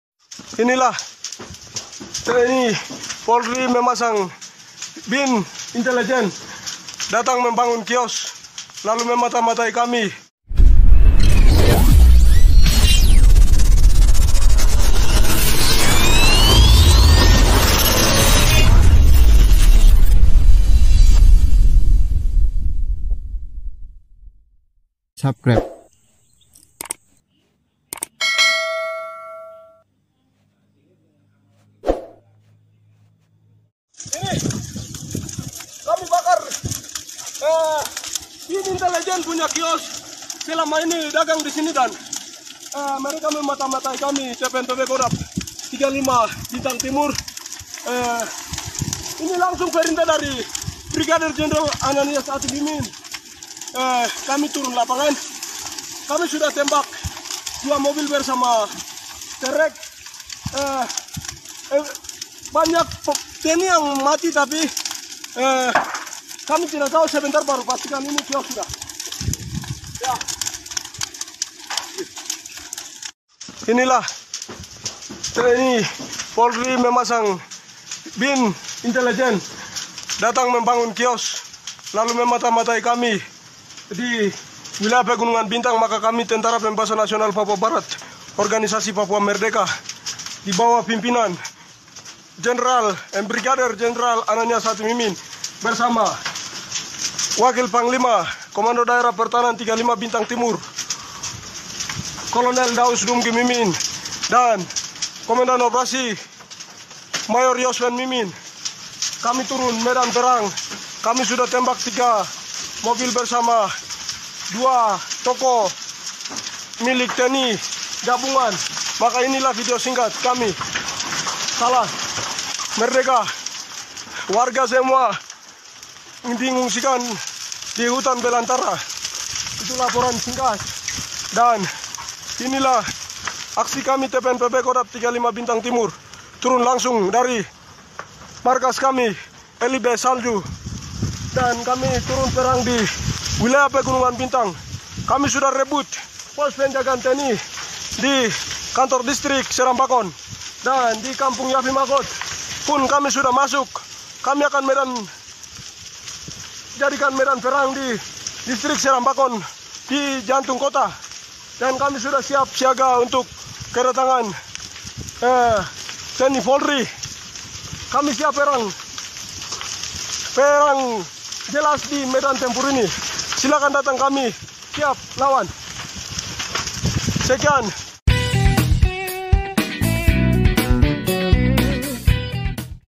KKB Kembali Berulah - Tembak 2 Warga Sipil dan 1 Polisi di Oksibil Pegunungan Bintang Papua
Kelompok Kriminal Bersenjata (KKB) menembak 2 orang warga masyarakat di Kampung Okpol, Distrik Oksibil, Kabupaten Pegunungan Bintang, Papua Pegunungan, Senin malam, 18 September sekitar pukul 19.00 WIT.